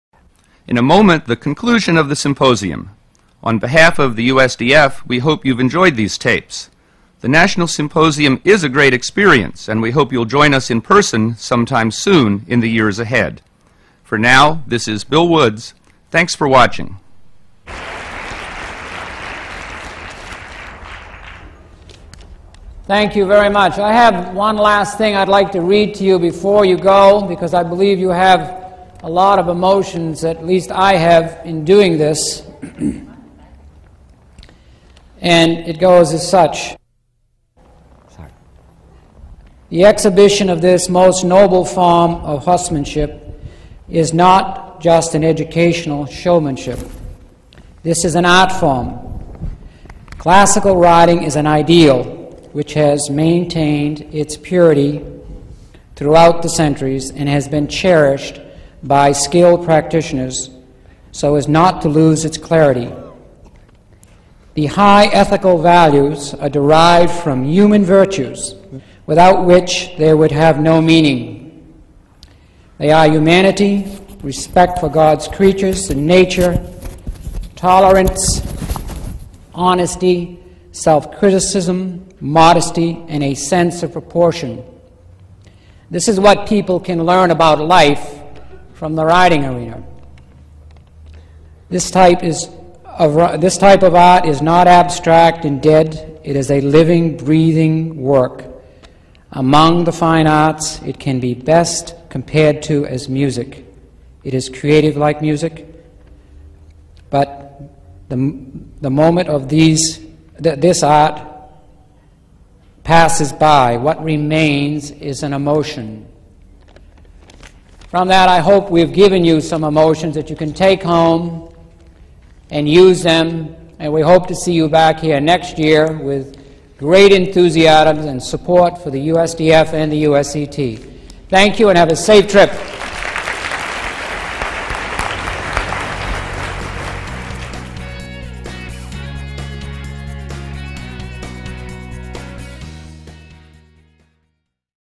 1996 USDF National Symposium: The Art of Dressage
1996 USDF National Symposium featuring Robert Dover, Michael Poulin, Hilda Gurney, and Jessica Ransehousen. Watch as Michael Poulin comments on the 'Art of Dressage' For more videos like this, visit the USDF Education Library